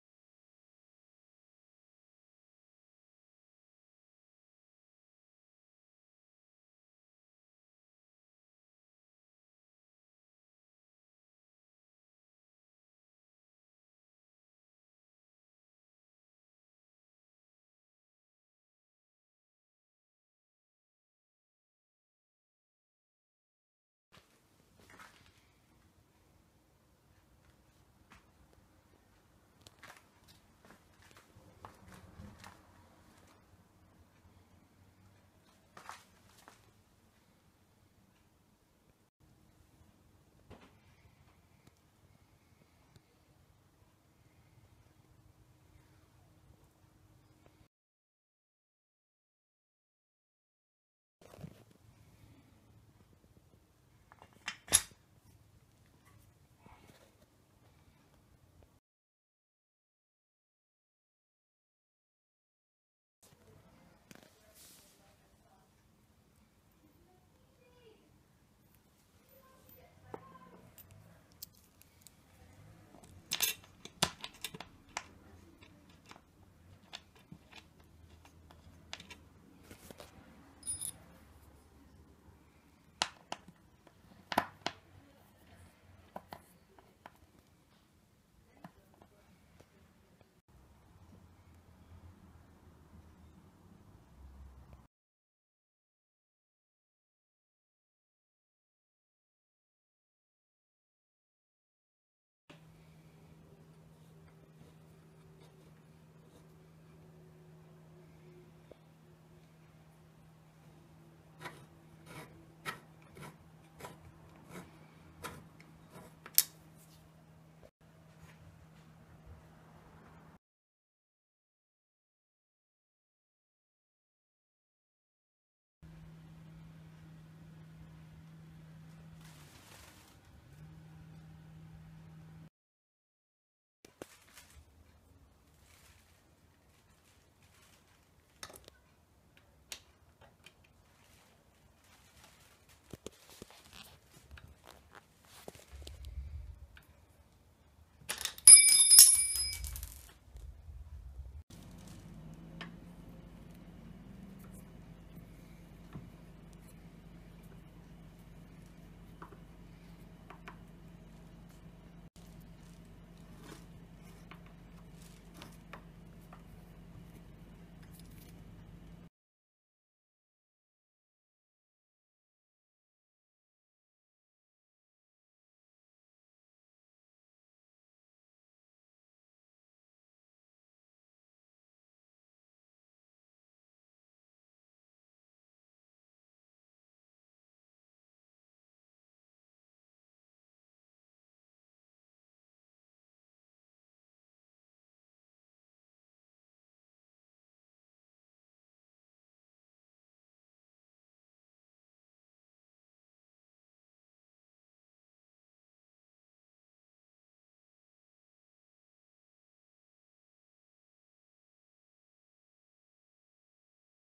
Assembling a desk chair
If you are considering getting a desk chair you will need to put it together. This is not too difficult. You will probably only need an Allen key to screw in some bolts, the Allen key is usually provided as part of the kit.
Assembling a Songmics chair